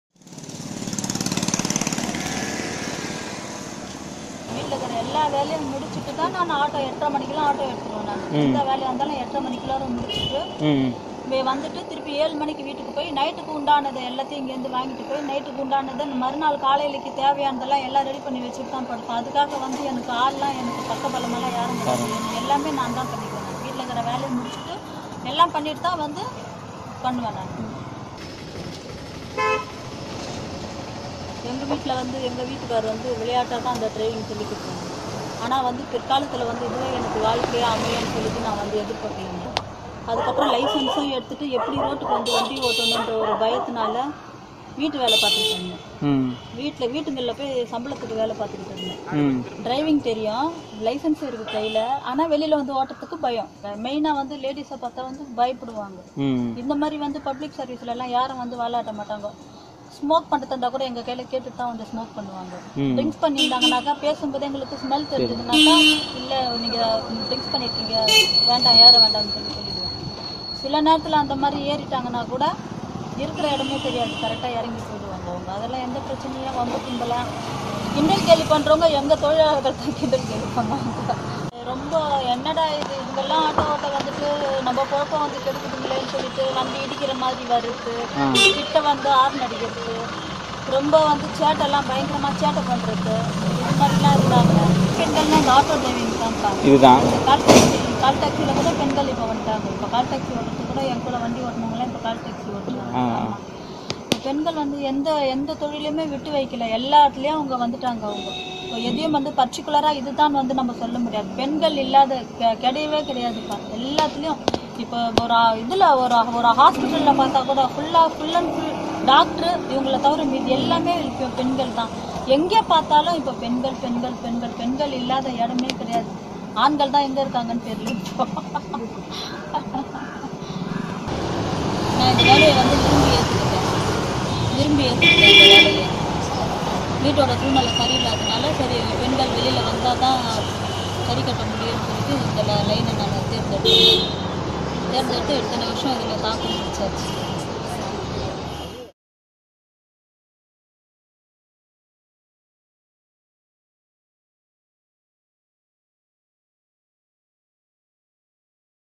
Against All Odds: The Auto Driver from TN
Community correspondent Mani Manickem reports from Tamil Nadu for Video Volunteers.